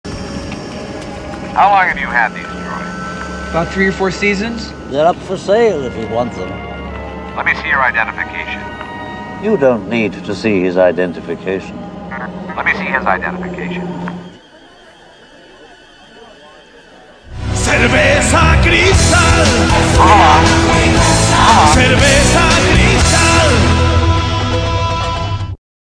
Obi-Wan bribes a stormtrooper with CERVEZA CRISTAL
Cerveza Cristal has a strong influence on the weak minded...

Enjoy my content?

Jungle in a Room Studios